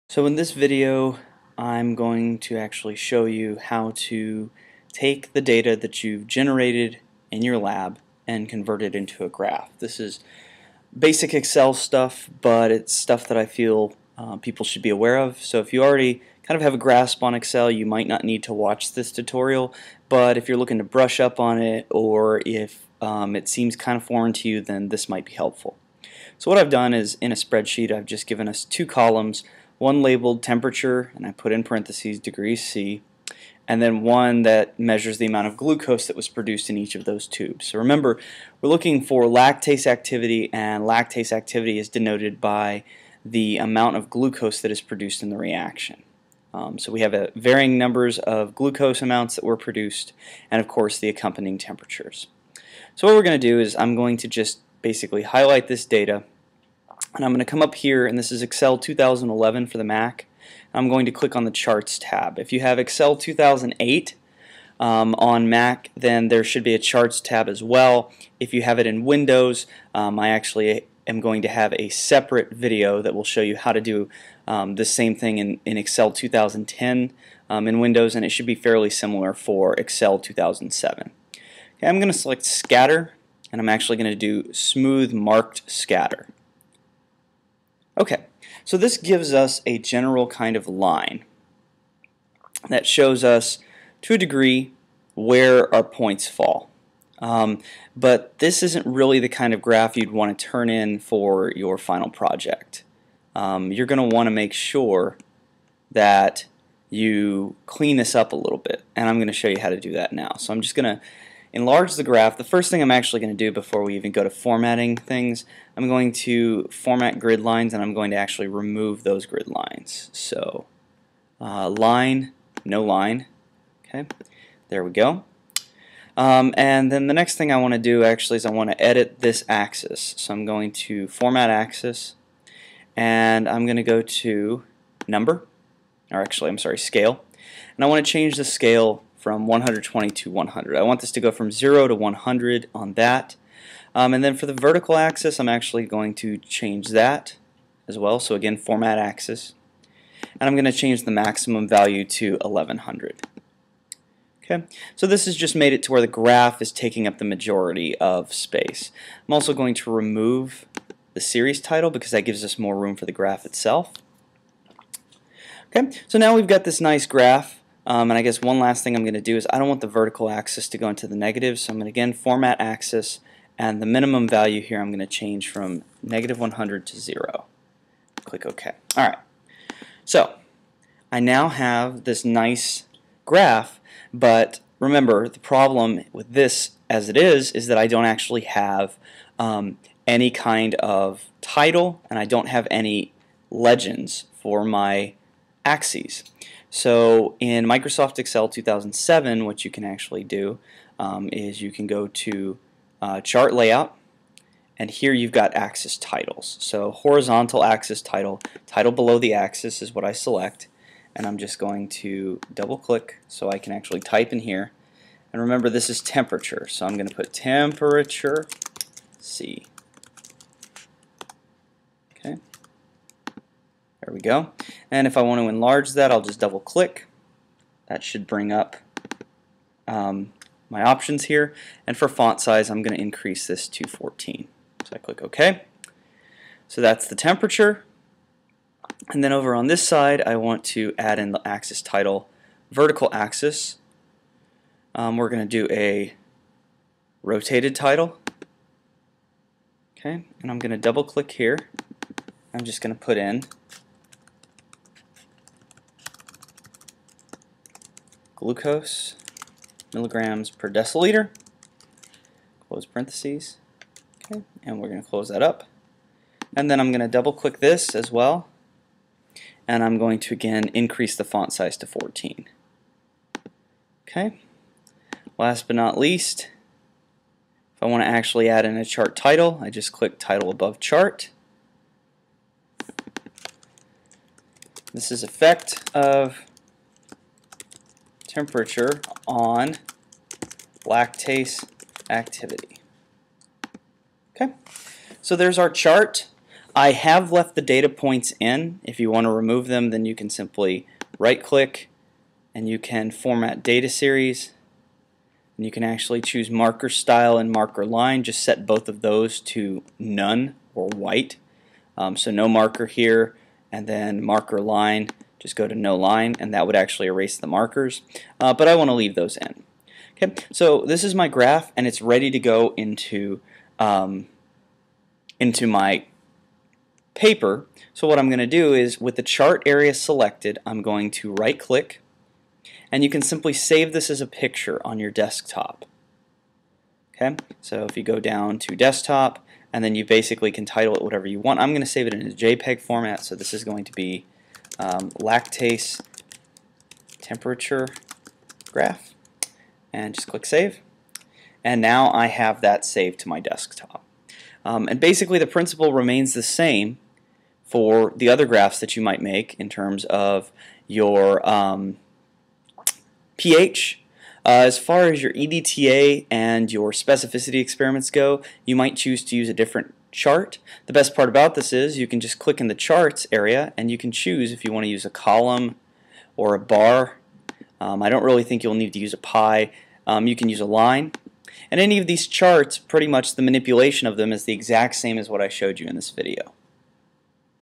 Office Tutorials - An Introduction to Graphing (Microsoft Excel 2011)
In this video I give you the basic concepts and instructions for creating a graph in excel.  In this way you can easily take your enzyme data and convert it into a variety of graphs that look clean and elegant.  You can find a collection of my iWork and MS Office tutorials